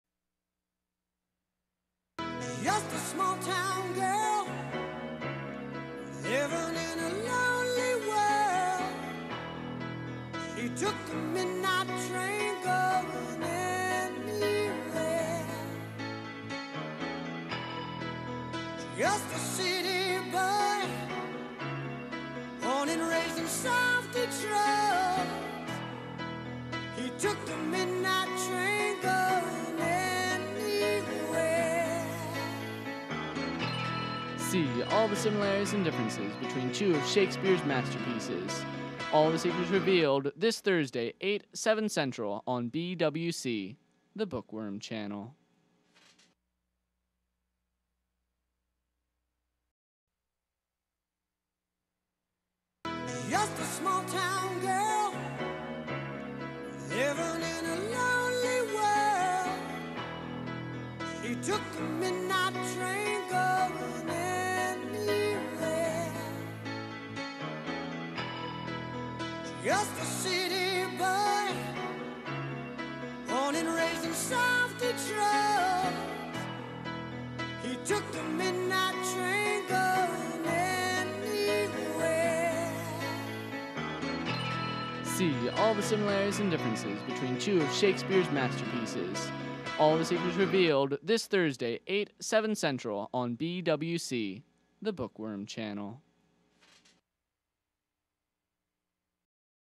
Shakespeare Unmasked
A trailer for a ficticious TV series.  An English Project.